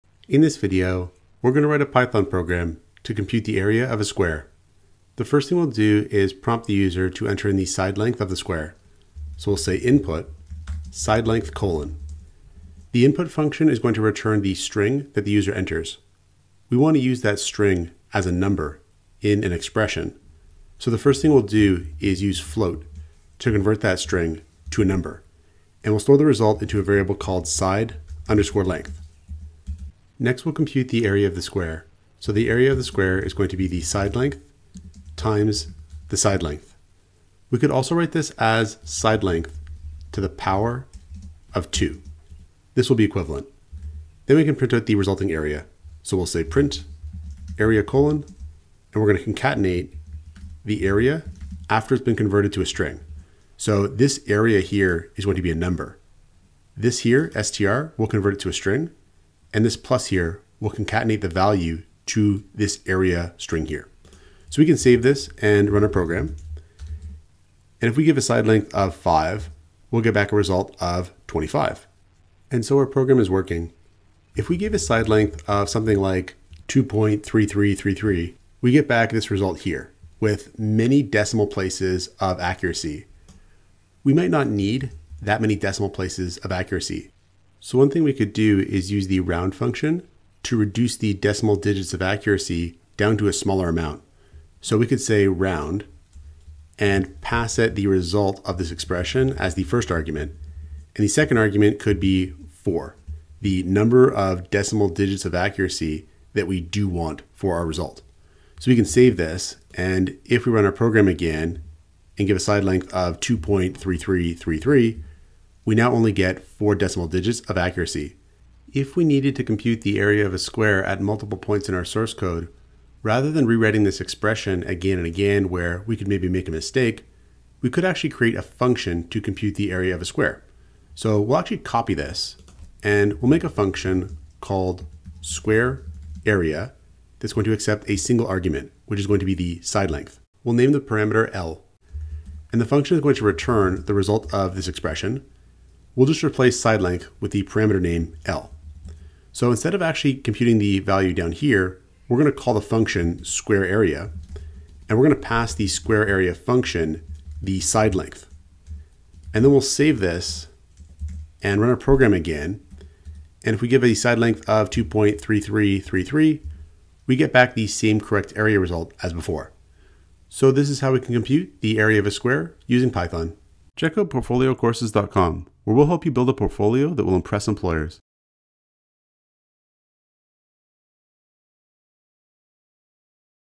Area of a Square | Python Example
How to compute the area of a square using Python.  Source code to build a portfolio that will impress employers!